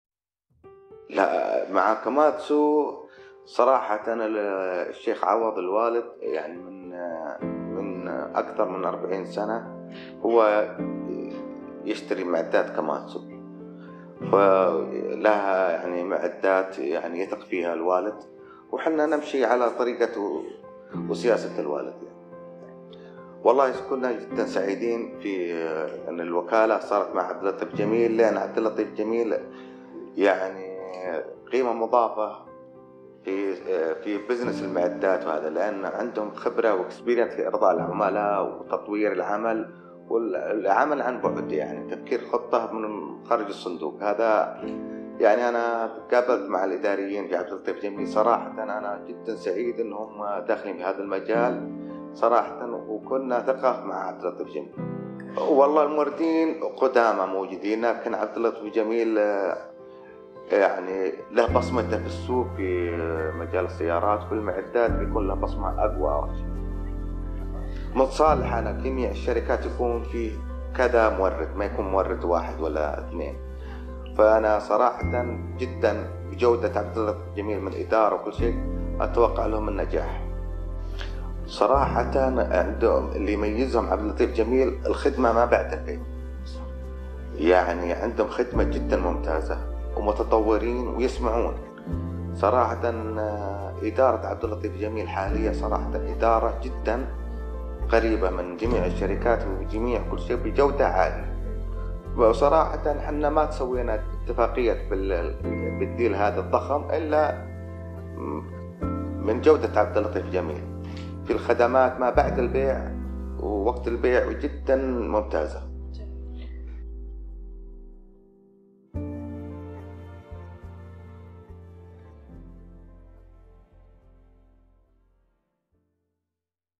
Voice of customer Bin Quraya Holding Company
One of the largest engineering, procurement and construction (EPC) companies catering to the energy sector in Saudi Arabia, Bin Quraya Construction, a part of Bin Quraya Holding Company, is a go-to partner for building energy production and distribution facilities across the country, primarily for Saudi Aramco.

"As we expand our geographical reach and undertake larger and more complex projects, we have the responsibility and need to offer the best equipment to our customers.” , Sheikh Hamad Bin Quraya, CEO, Bin Quraya Holding Company shared his thoughts on equipment selection.